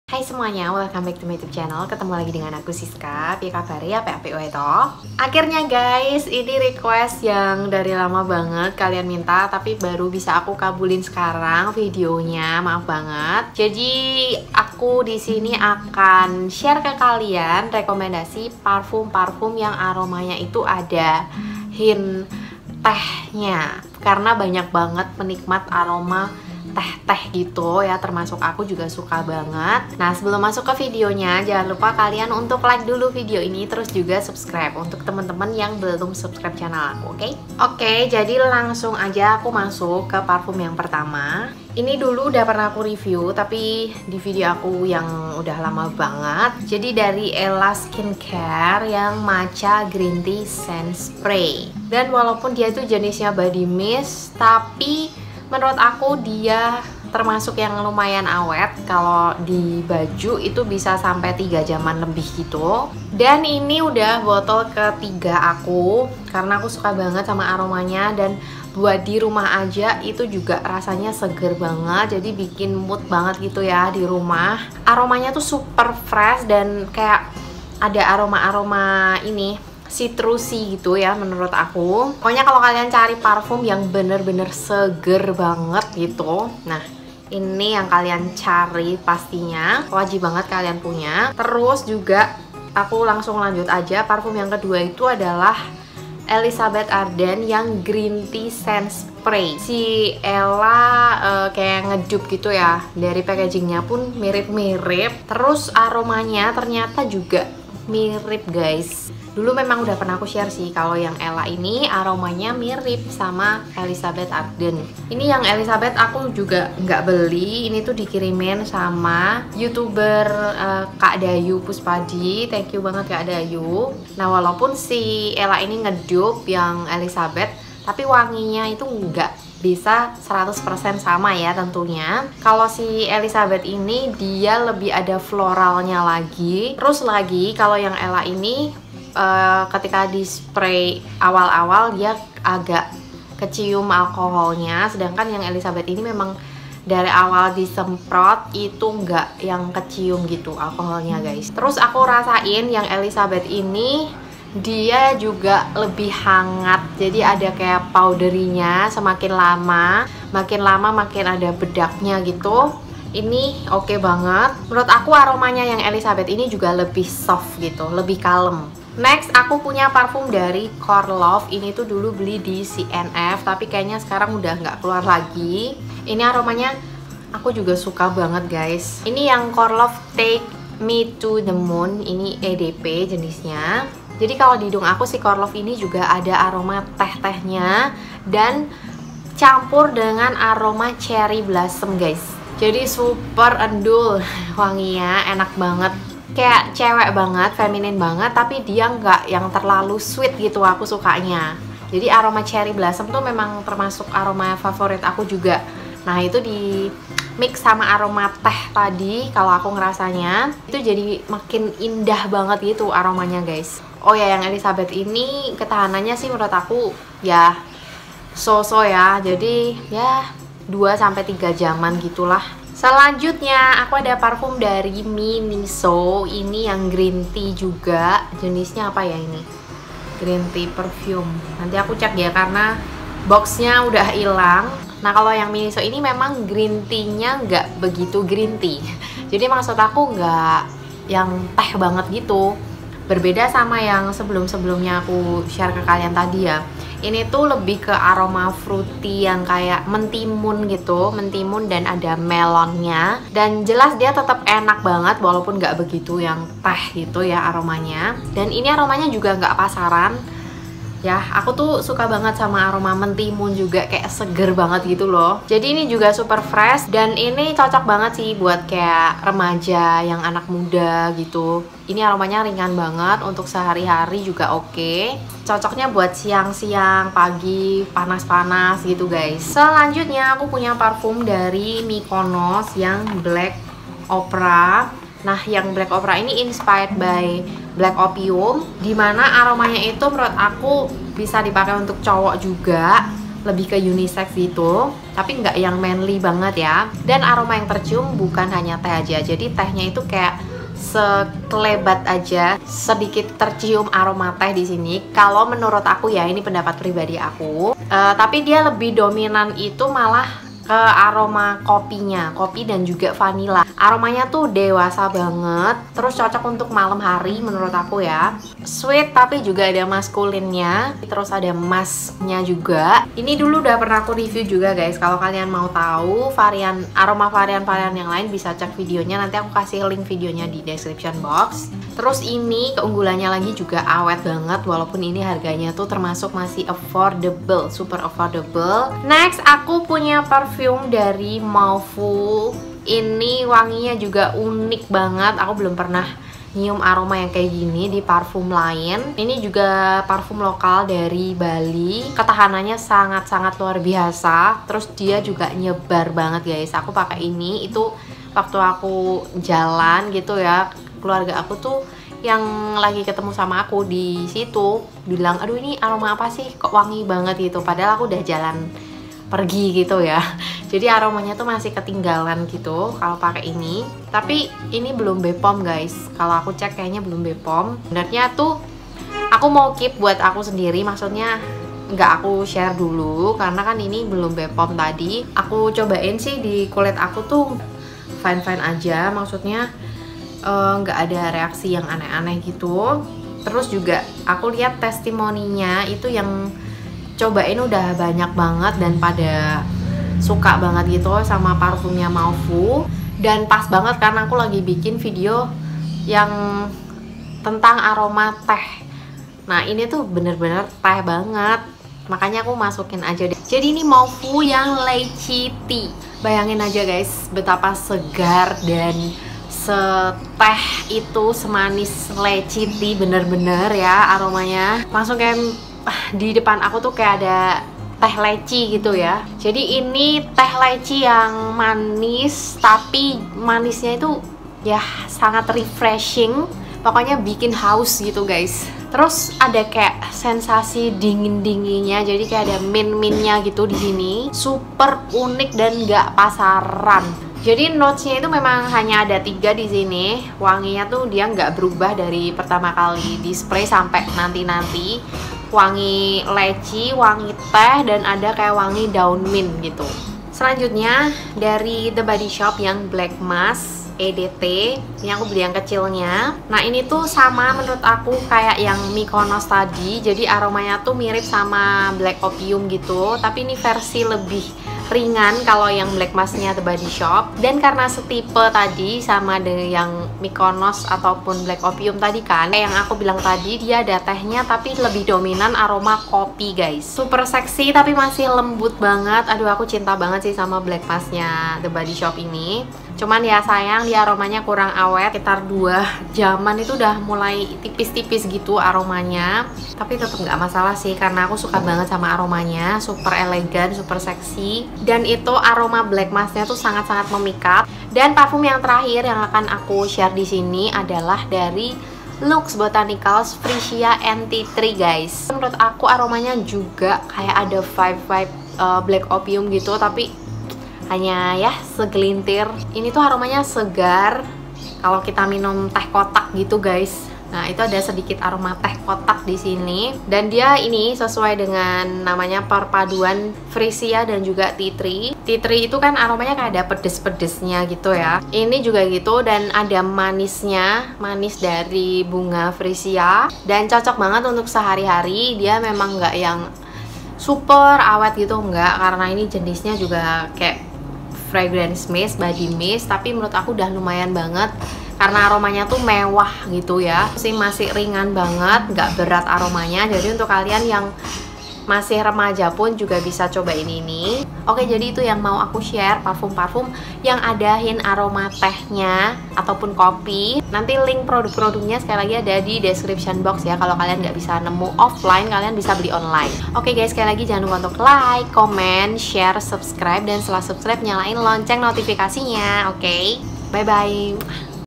8 PARFUM AROMA TEH TERBAIK! PARFUM WANGI GREEN TEA, LECI TEA,JASMINE TEA. UNIK2!
Rekomendasi parfum aroma Teh terbaik! Yang kalian request terus nih guys hehe
Parfum wangi Teh yang enak banget dan ada yang super tahan lama sampe aromanya ketinggalan! Nyebar banget dan tercium dari jarak jauh.